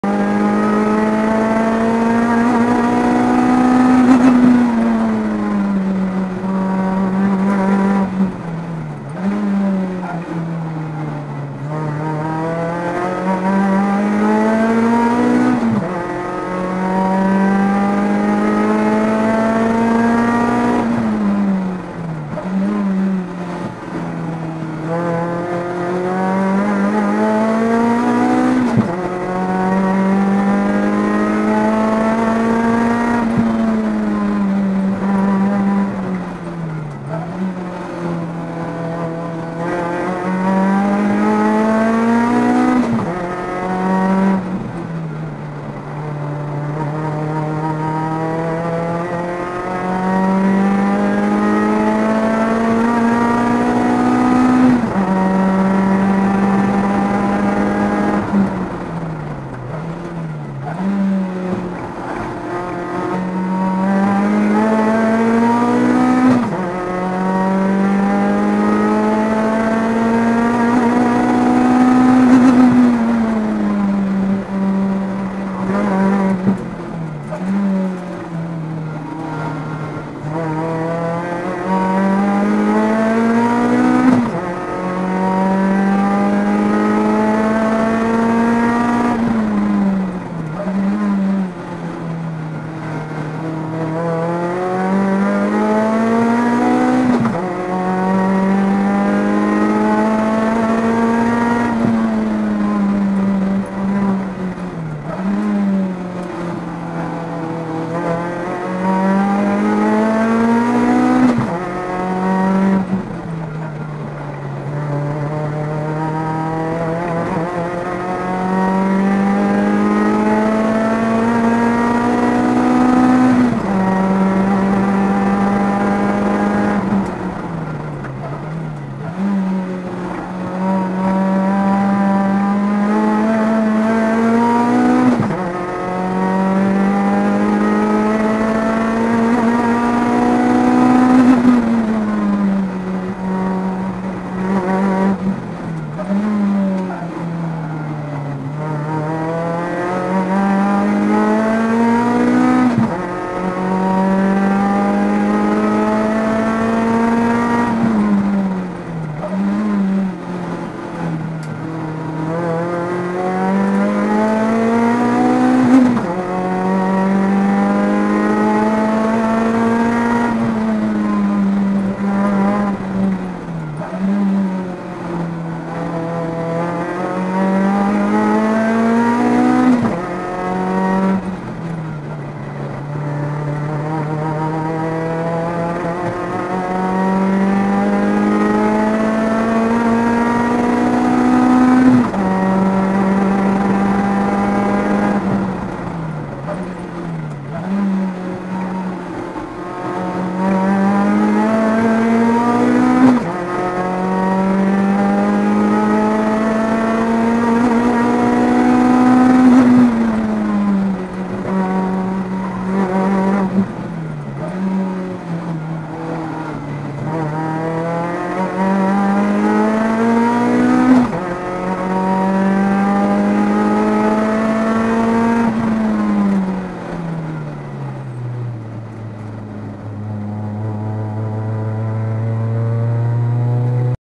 AW11 MR2 4agze Rotrex / Megara # 1'08.234"
HTTC Track day 03/10/15
- Rotrex
- Dunlop D03G Semi 215/50R15
- Stock MK1a brakes, yellow stuff rear, CL RC6 front
- Air scoop, fastback